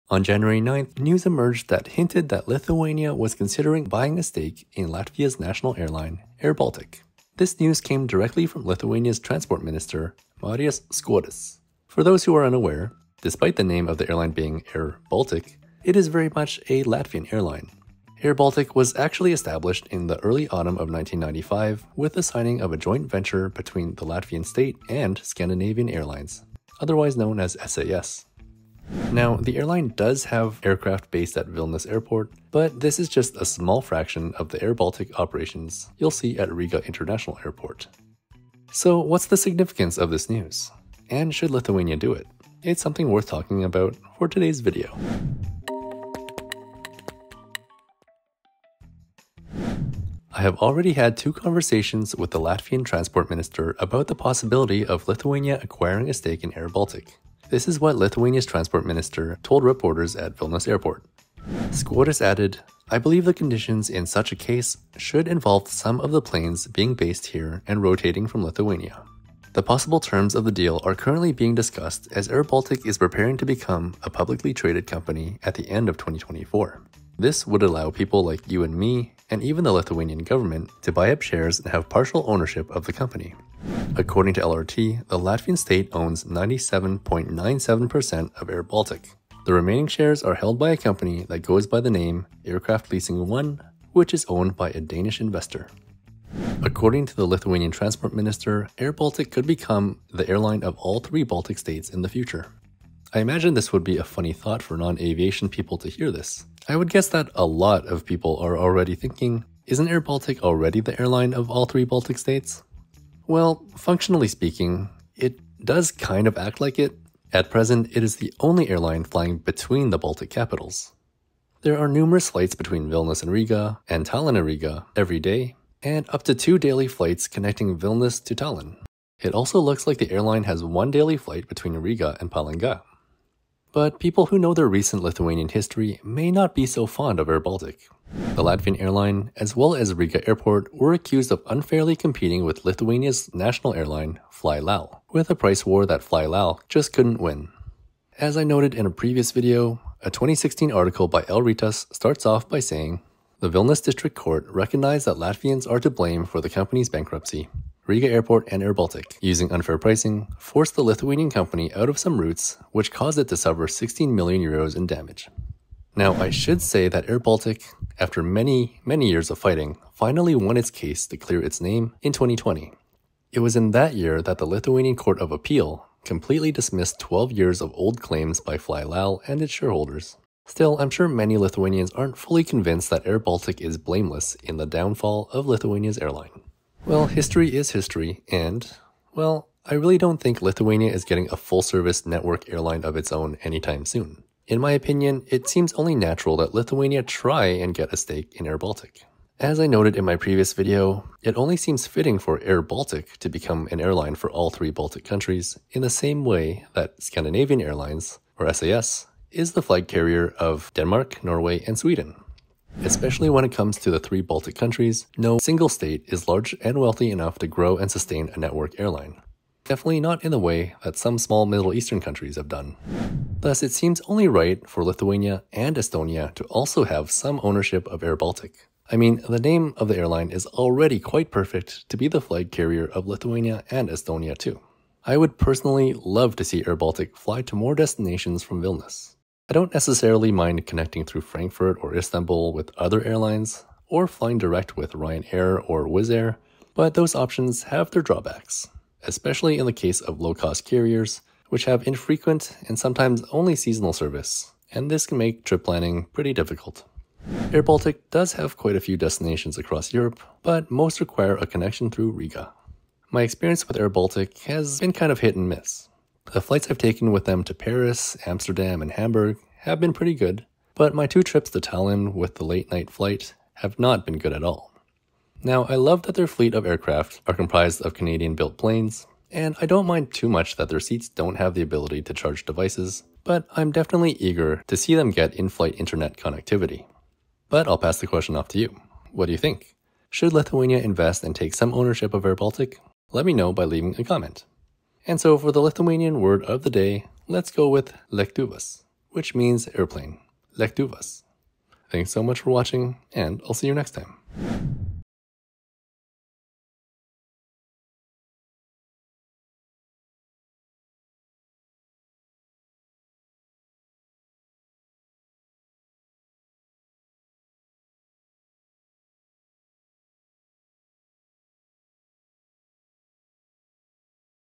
Should Lithuania Buy Shares Of Latvia’s Airline, airBaltic?
On January 9th, news emerged that Lithuania was considering buying a stake in Latvia’s national airline airBaltic. This news came directly from Lithuania’s Transport Minister Marius Skuodis. Should the country do it?